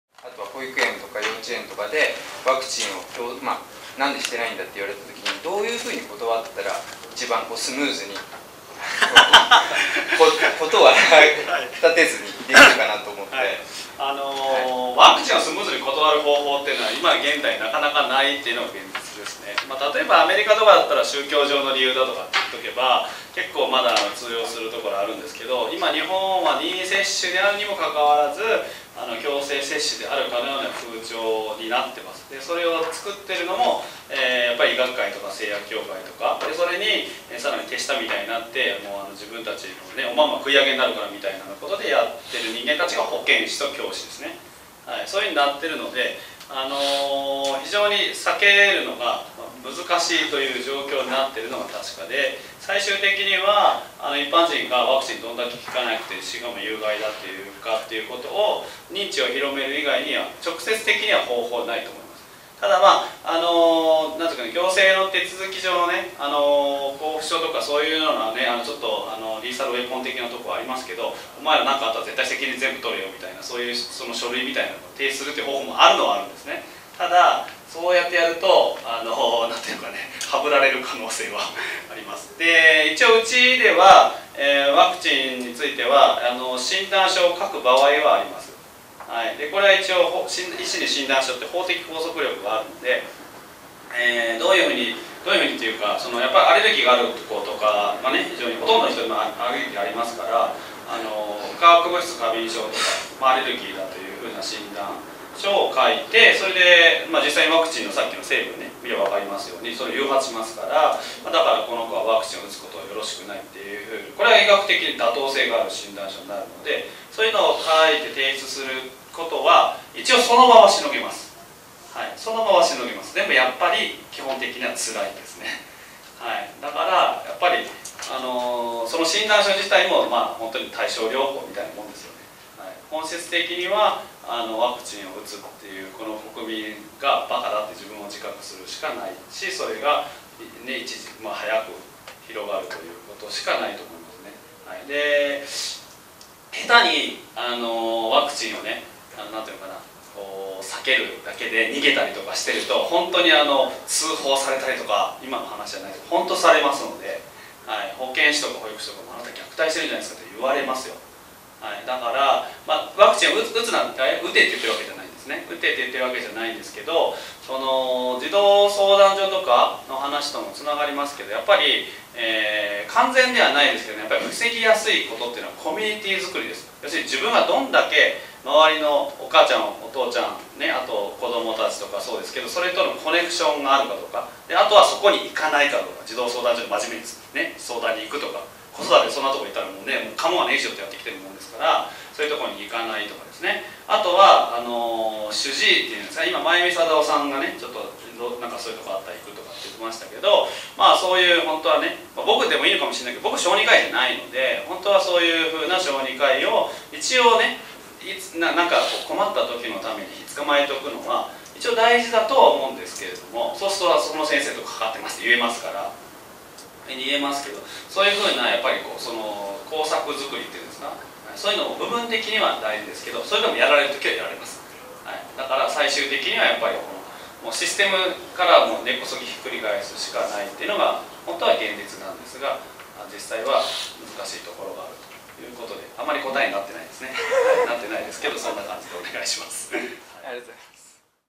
【動画】『子どもを守る為に ～ワクチンが危ない！児童福祉の怖い話～』Q&A「ワクチン」編
『子どもを守る為に ～ワクチンが危ない！児童福祉の怖い話～』Q&A「ワクチン」編
よくわからないこと、聞いてみたいこと
「ワクチン接種をスマートに逃れるには？」
Tokyo DD Clinic院長　内海 聡先生にお話しいただきました。

日時：2014年7月29日　火曜日
場所：神奈川産業振興センター13階 第二会議室
講演： 内海 聡（Tokyo DD Clinic院長）× 矢野美奈（Happy Mommy Daddy代表）
主催：子育てサークHappy Mommy Daddy